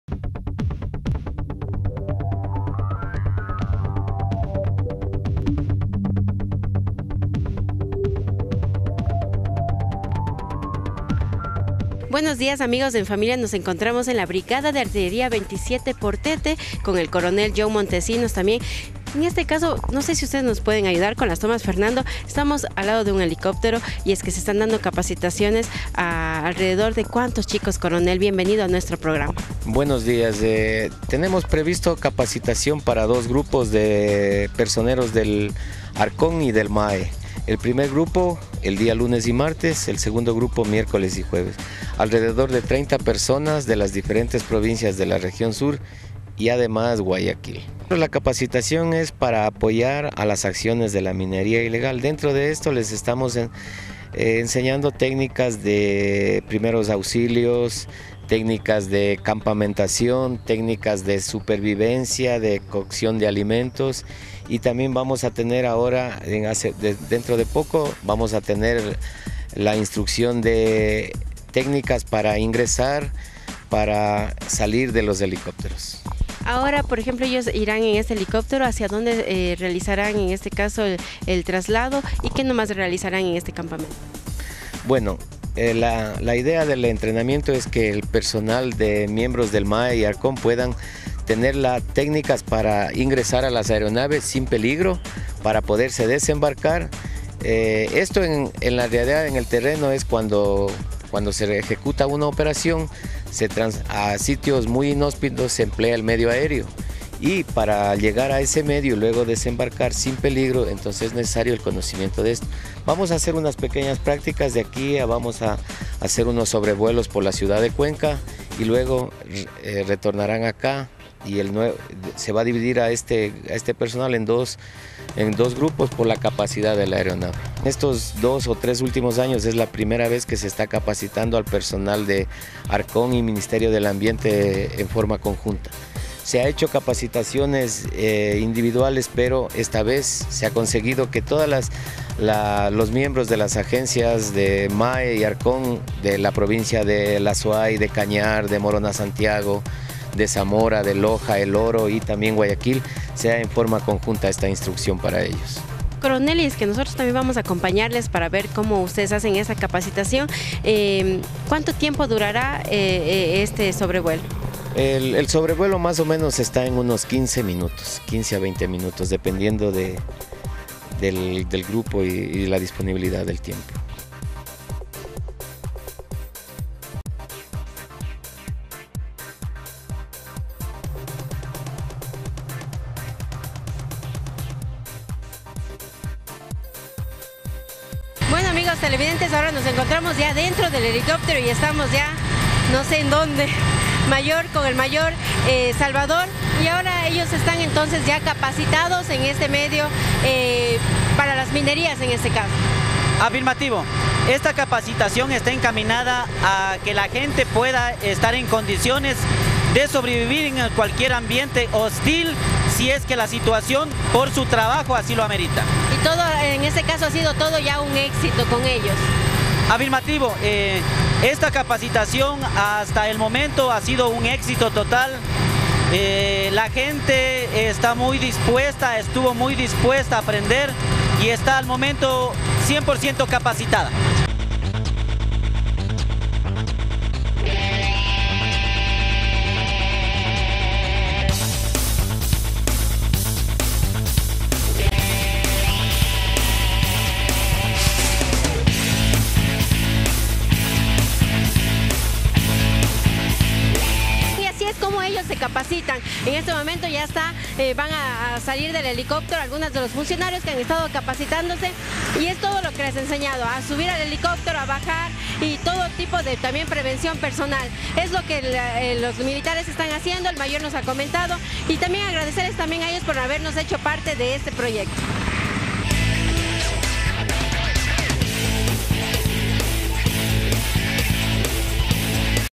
Enfamilia ARCOM Capacita a funcionarios
María José Ortega fue parte de esta capacitación con la ARCOM